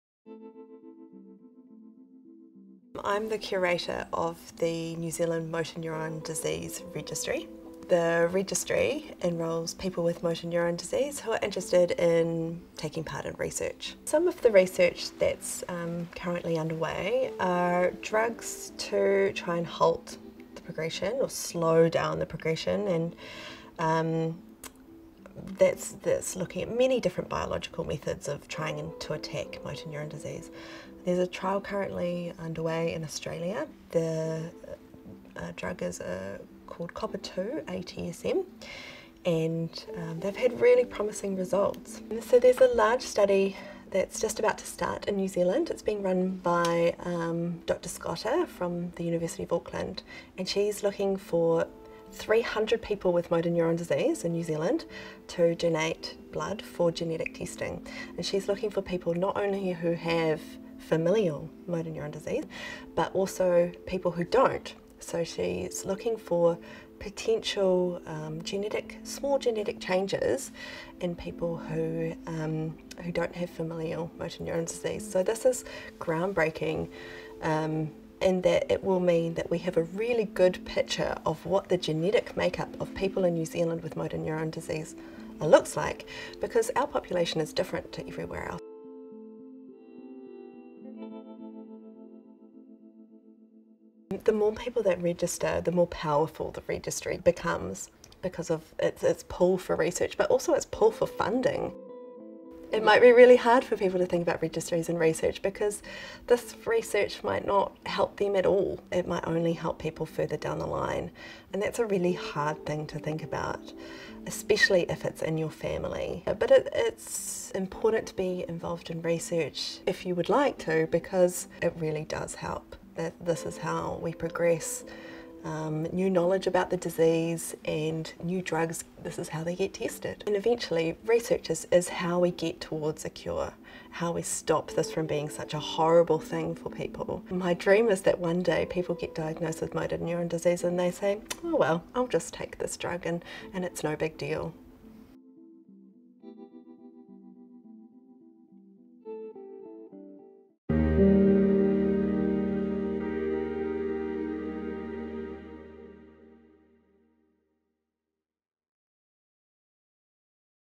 Finding a Cure for Motor Neurone Disease
MND Registry Curator, Kerry Walker dreams that one day people diagnosed with motor neurone disease are able to say “oh well I'll just take this drug and it's no big deal.”

New research is underway to find that cure.